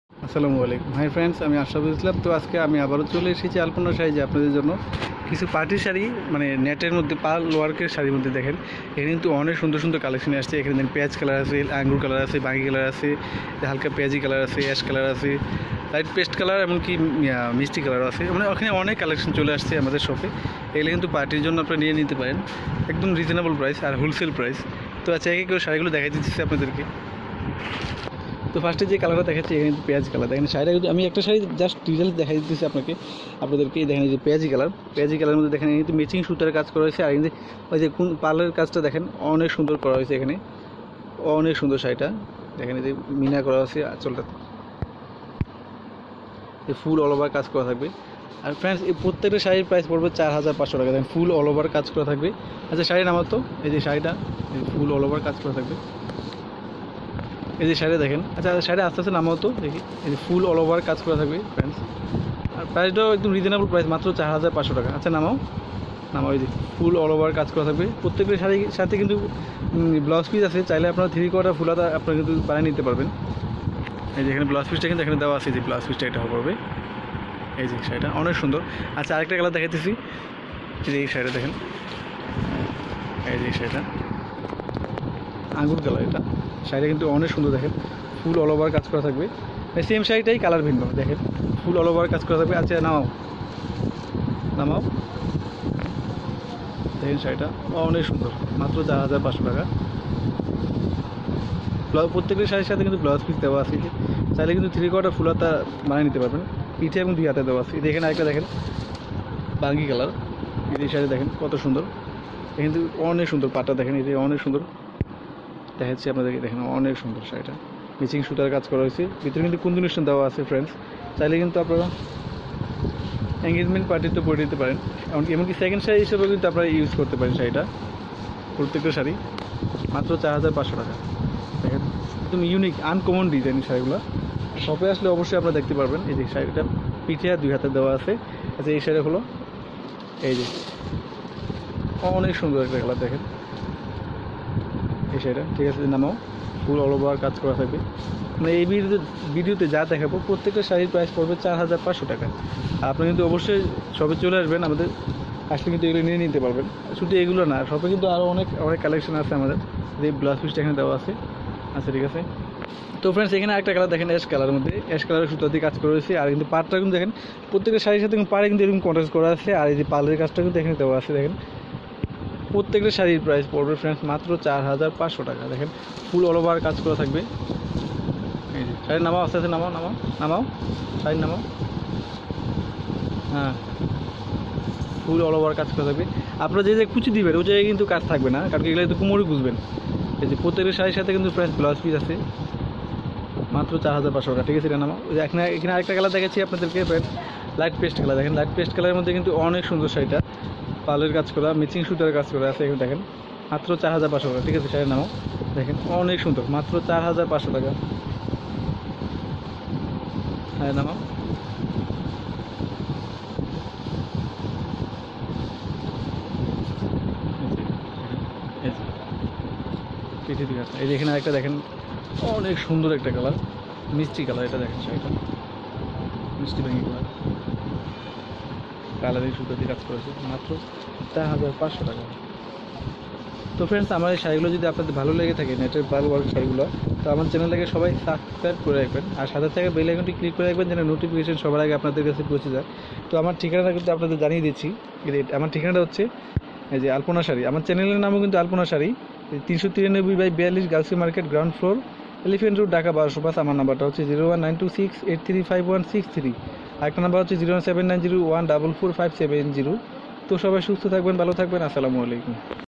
পাইকারি দামে ইন্ডিয়ান গুজরাটি নেটের পালি শাড়ি কিনুন (আলপনা শাড়ি)গাউছিয়া মার্কেট থেকে
আলপনা শাড়ী,গাউছিয়া মার্কেট (নিচ তলা) এলিফ্যান্ট রোড, ঢাকা-১২০৫
মোঃ আশরাফুল ইসলাম